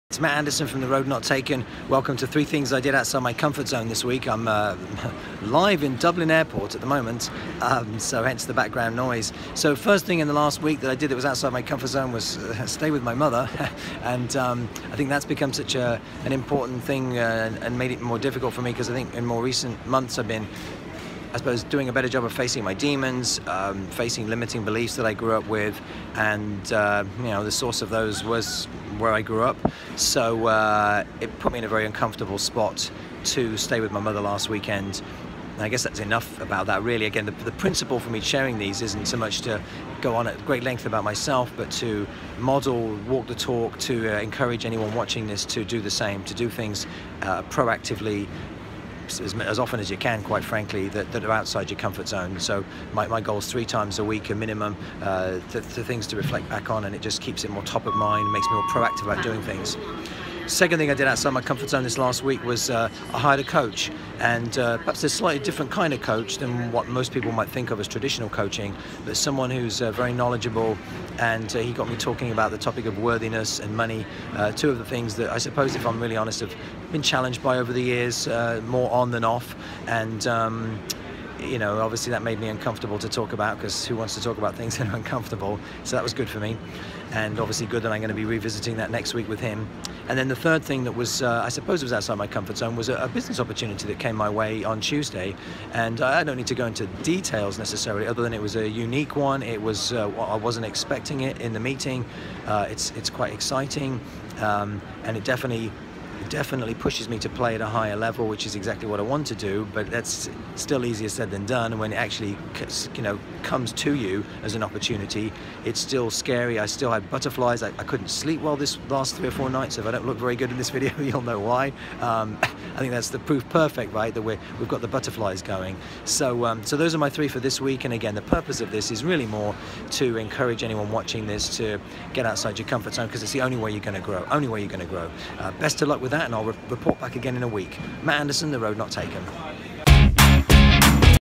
3 Things I Did Outside My Comfort Zone Week 4
I am also on the journey to get unstuck and take bigger leaps. Here are my 4 uncomfortable things from the past week, what were yours?